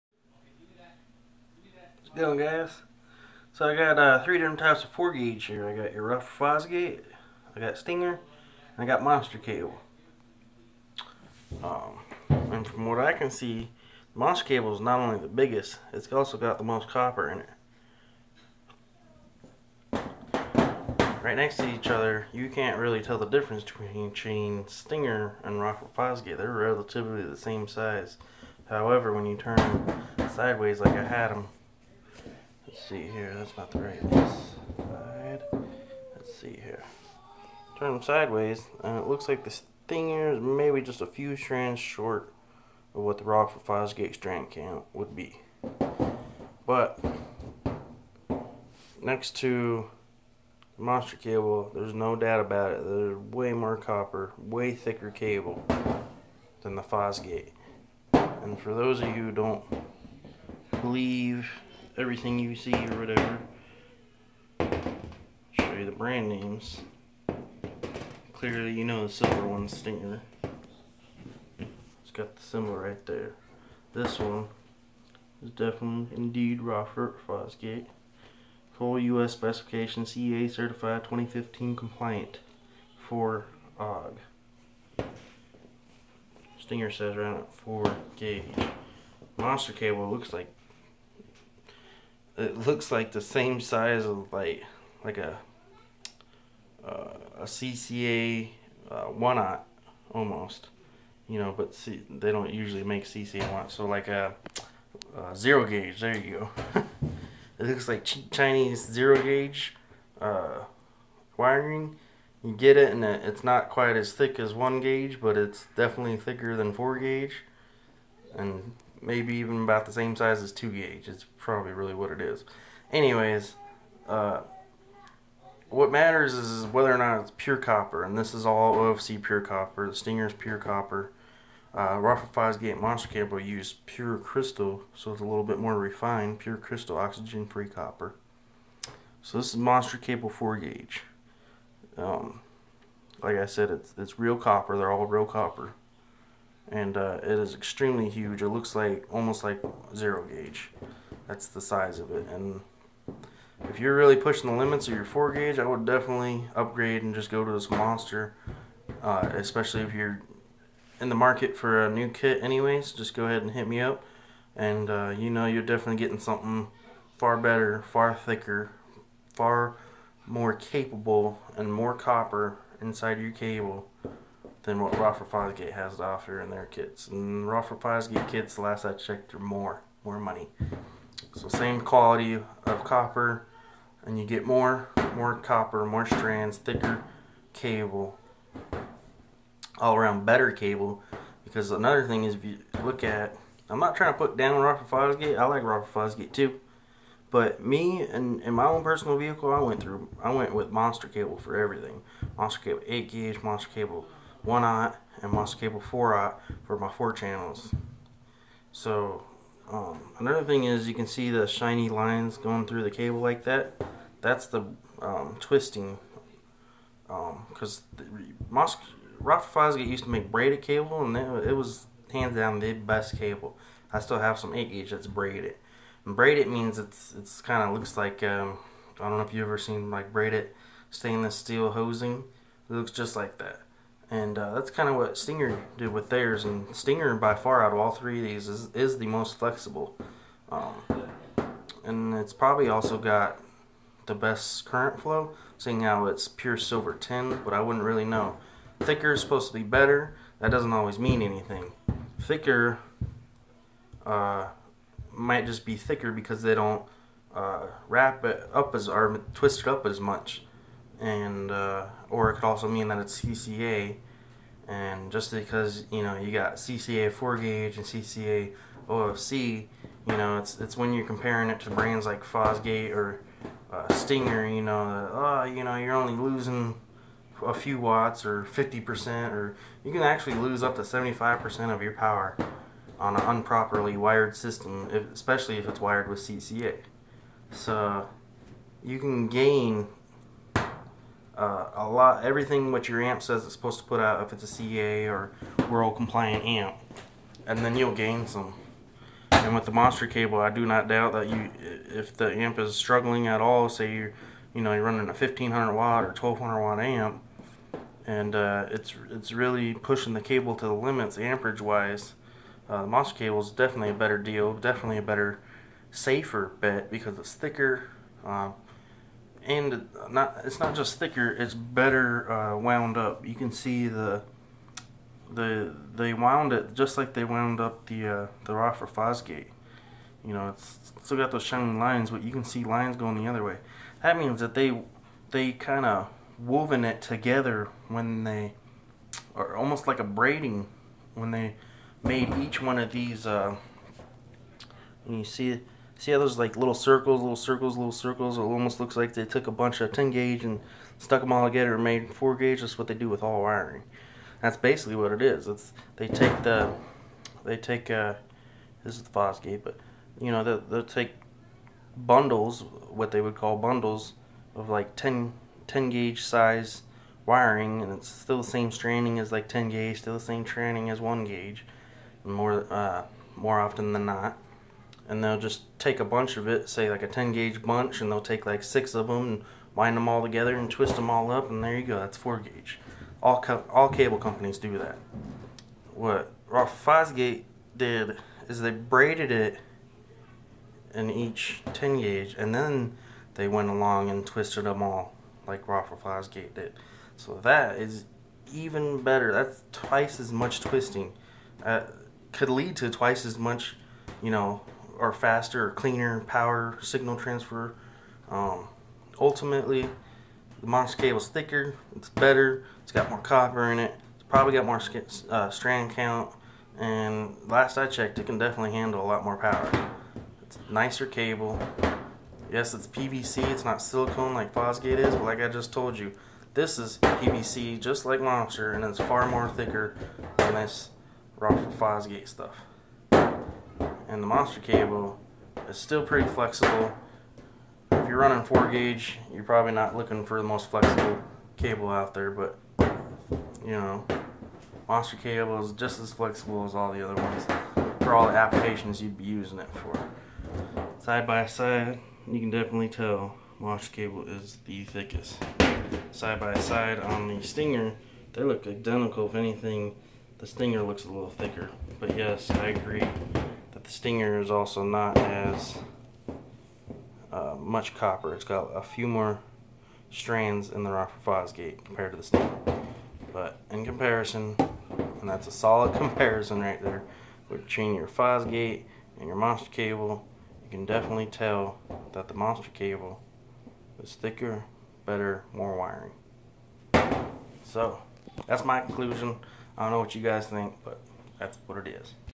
Rockford fosgate,Stinger,Monster cable ,4awg/4gage wire comparison
Showing the difference between the Rockford fosgate PC-OFC 4awg

Vs

Stinger pure silver tinned OFC 4awg

Vs

Monster cable PC-OFC 4awg